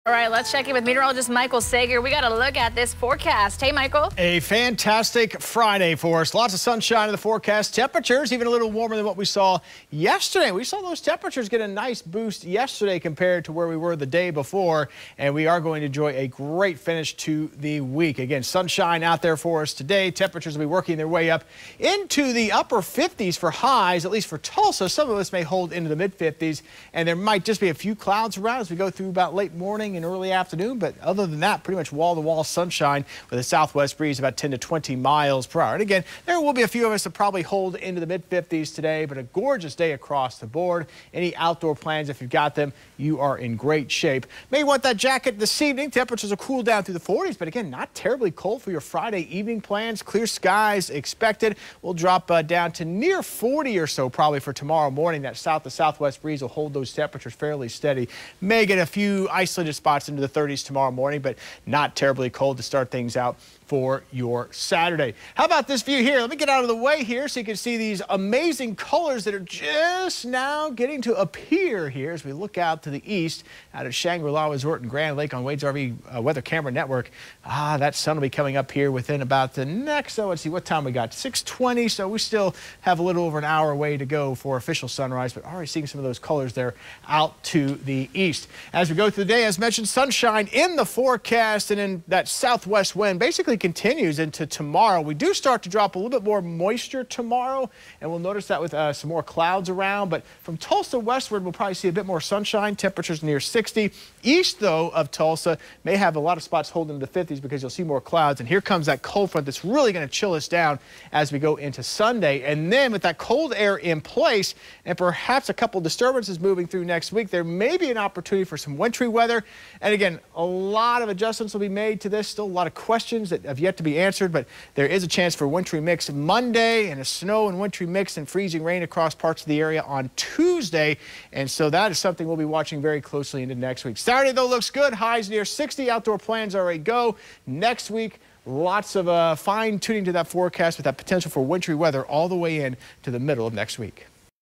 Fantastic Friday to finish the week
Cold front arrives Saturday night; Monitoring possible wintry weather next week